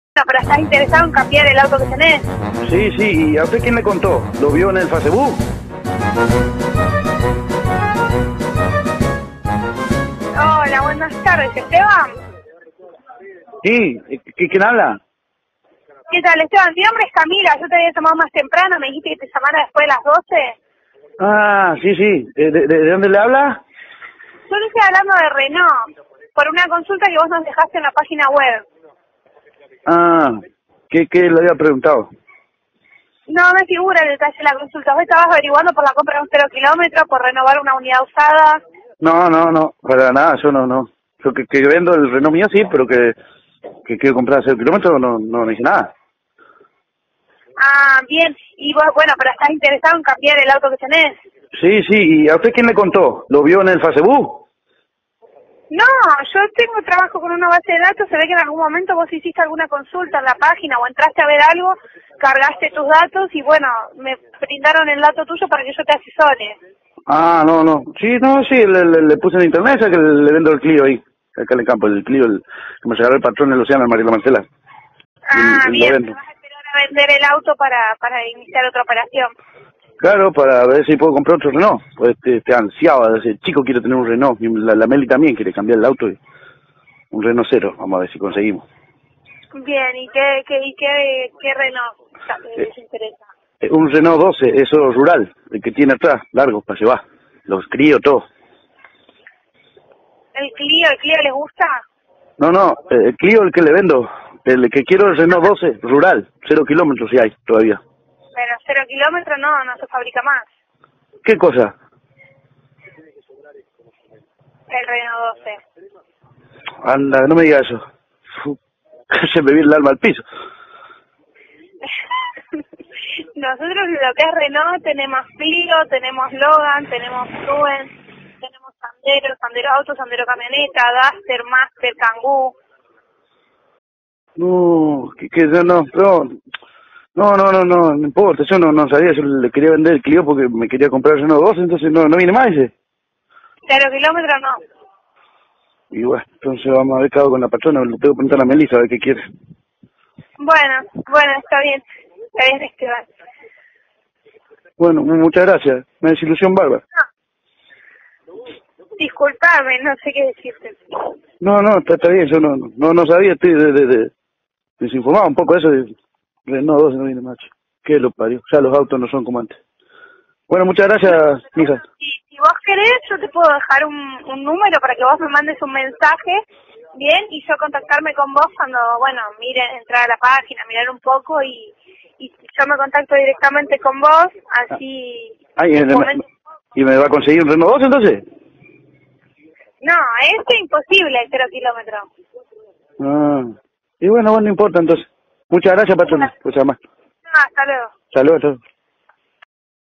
¿Qué auto quiero? Un Reno 12 rastrojero 0km
Esteban quiere un Reno 12 rastrojero 0km, y lo llaman para venderselo.